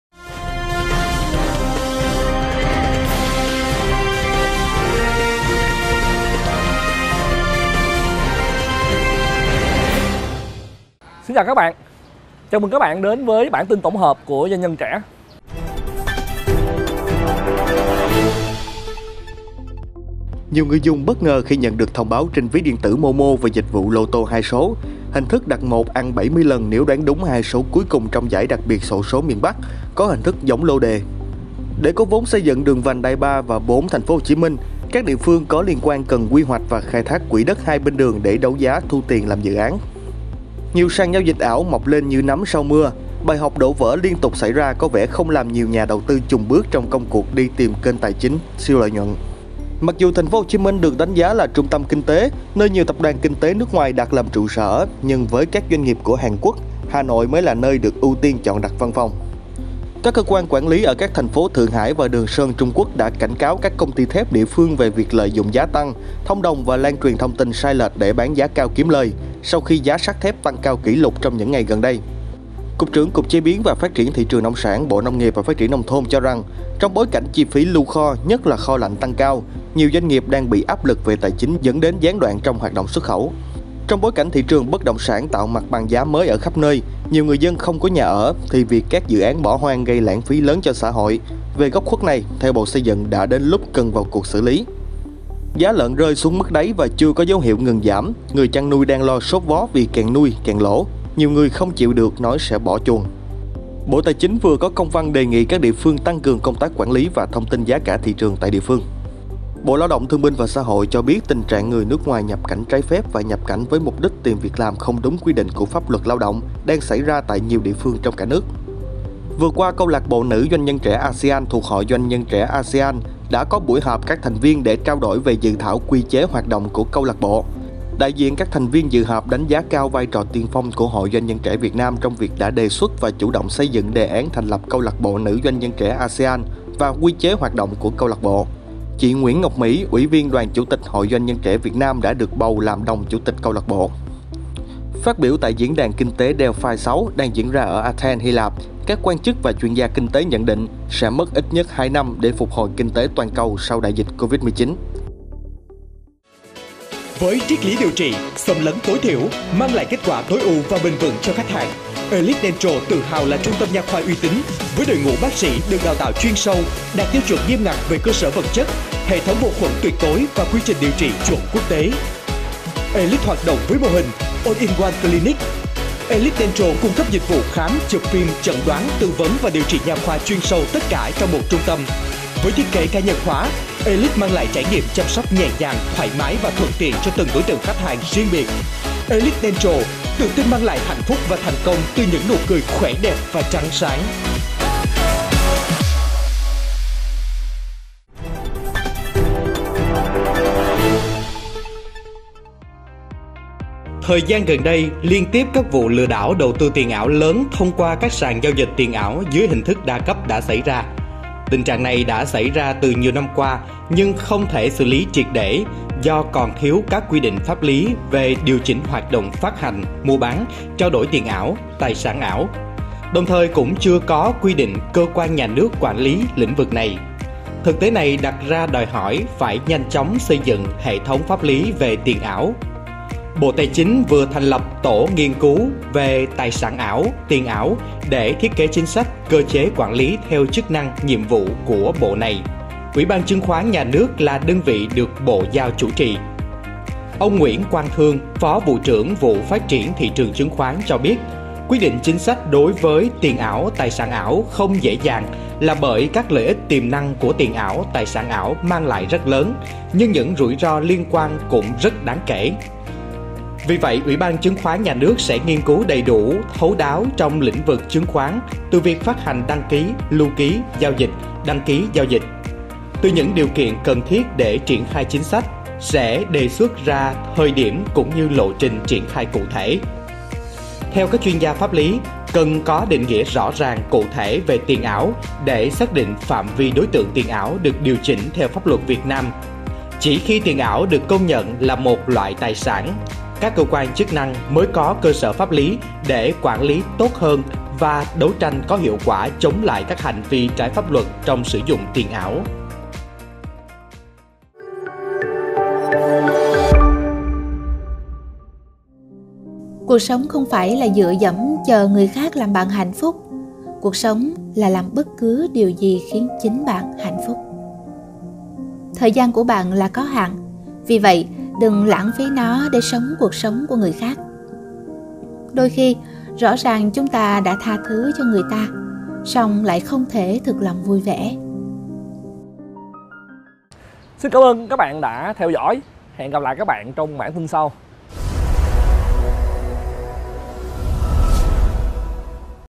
Bản tin tổng hợp (ngày 15/5)
Theo Bộ Xây dựng, đã đến lúc cần vào cuộc xử lý các dự án bỏ hoang; đại diện Hội DNTVN được bầu làm Đồng Chủ tịch CLB Nữ DNT ASEAN; sẽ mất ít nhất hai năm để phục hồi kinh tế toàn cầu sau đại dịch Covid-19,... Là những thông tin chính hôm nay